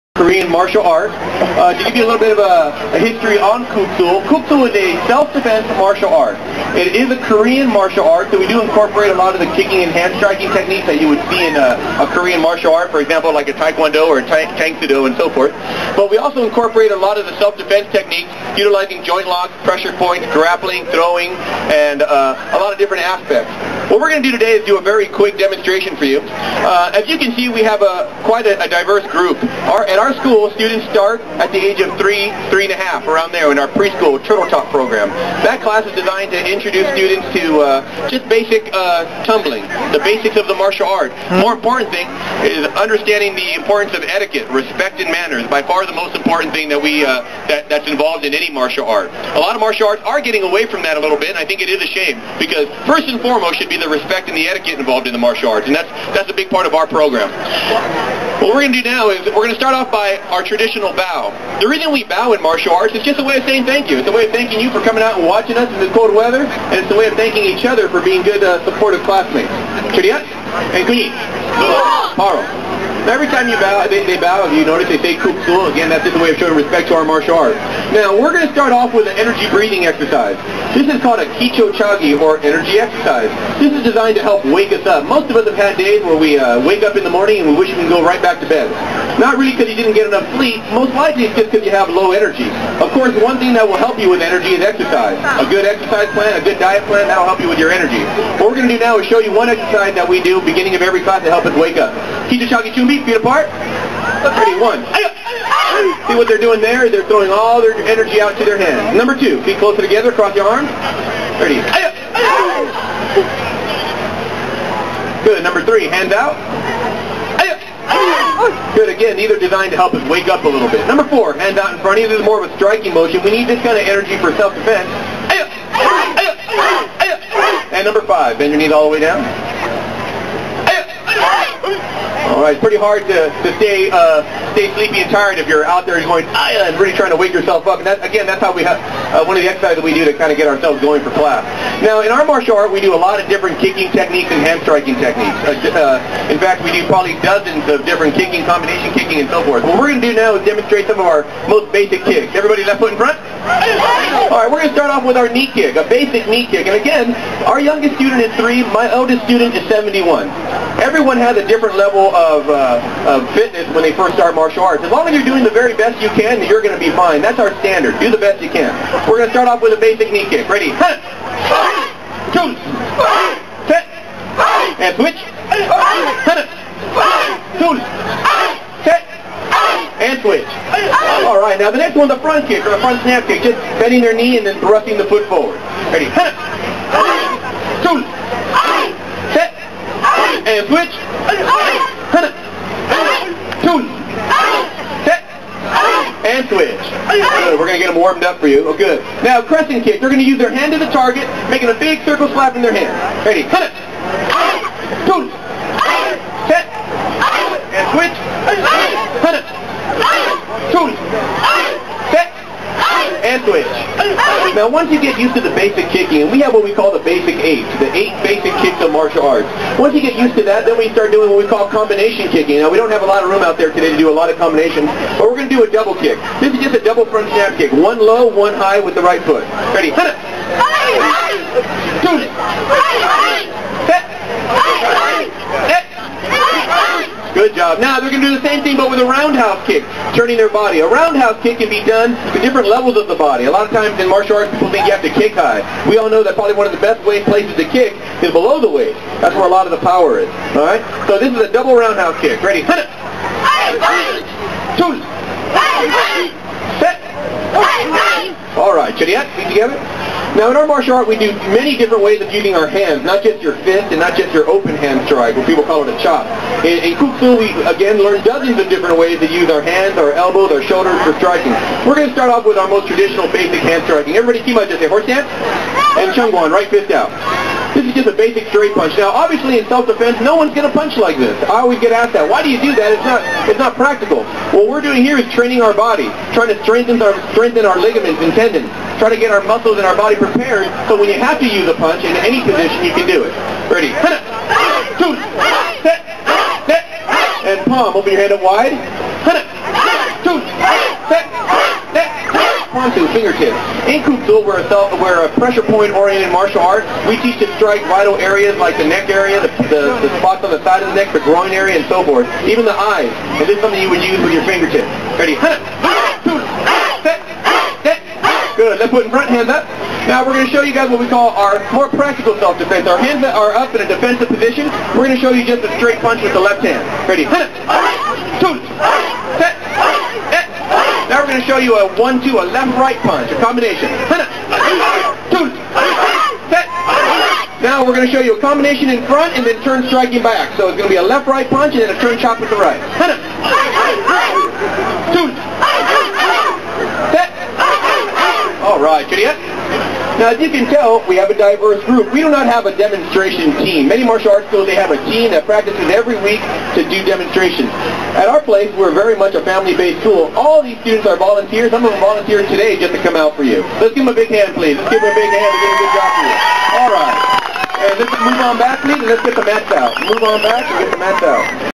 Kuk Sool demo Dec. 5, 2008
Daniel Tejada's Kuk Sool Won students demonstrate their skills during the Christmas Stroll on Plumas Street in Yuba City.